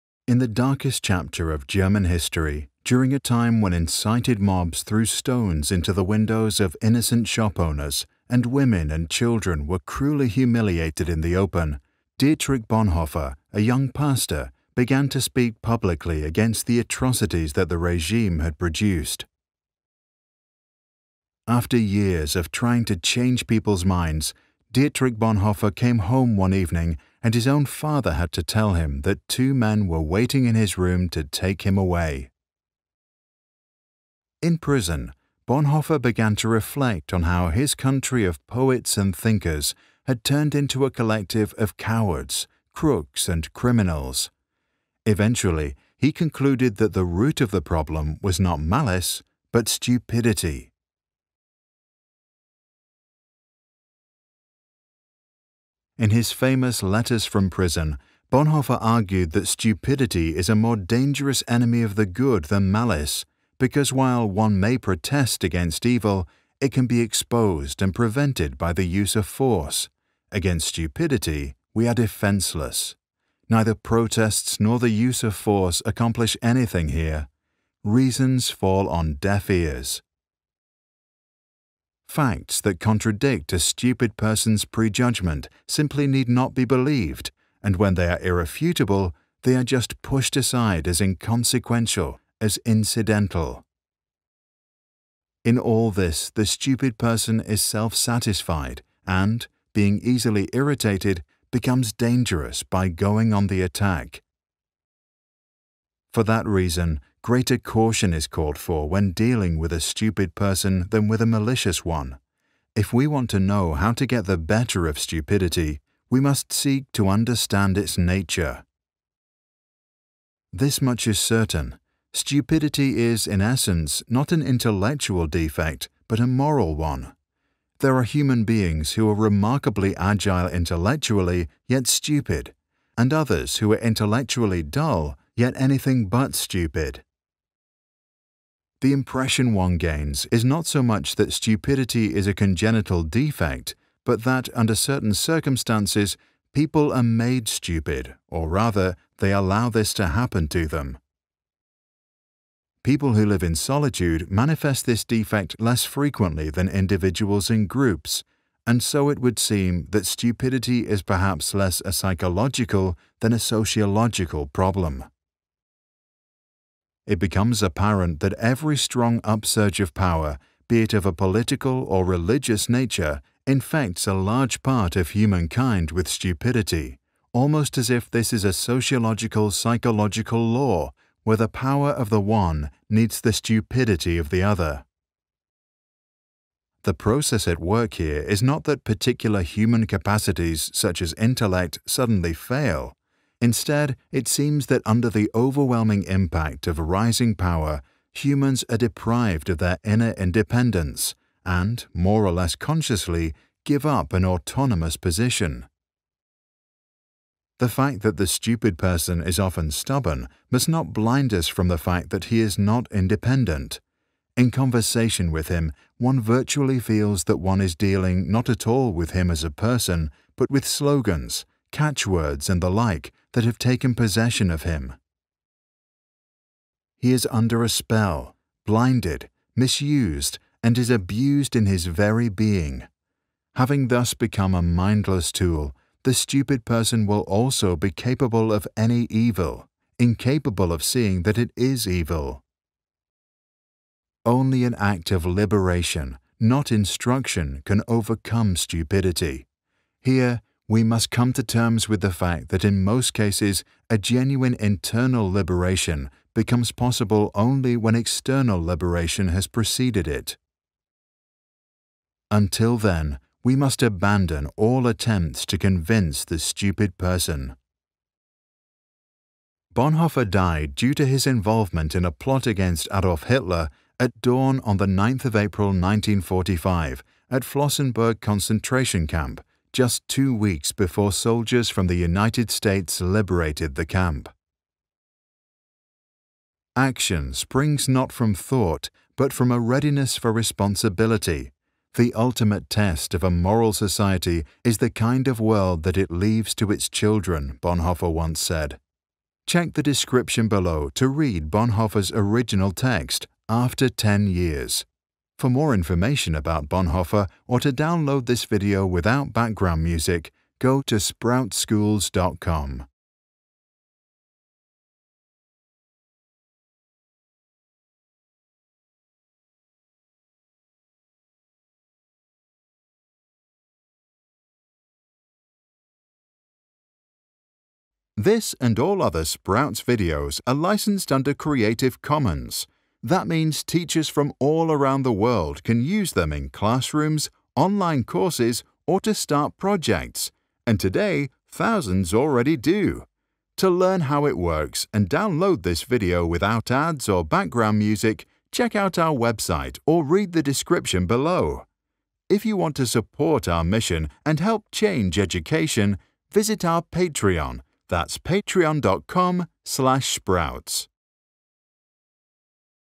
نظریه دیتریش بون‌هوفر درباره حماقت اجتماعی - زیرنویس فارسی
دیتریش بونهوفر،‌ فیلسوف آلمانی قرن بیستم، استدلال می‌کرد که در یک جامعه قدرت‌گرفتن افراد احمق از قدرت‌گرفتن افراد شرور خطرناک‌تر است؛ چون می‌توان علیه افراد شرور برخاست و با آنها مبارزه کرد، اما در برابر احمق‌ها دست‌‌ها بسته است، آنها فقط شعارها و مزخرفات خودشان را طوطی‌وار تکرار می‌کنند و با هیچ استدلالی قانع نمی‌شوند.
در این ویدئو رساله معروف بونهوفر درباره حماقت با کمی ویرایش ارائه شده. متنی که در خدمت جامعه آزاد و هشداری است درباره آنچه وقتی در جامعه‌ افراد خاصی قدرت می‌گیرند ممکن است اتفاق بیفتد.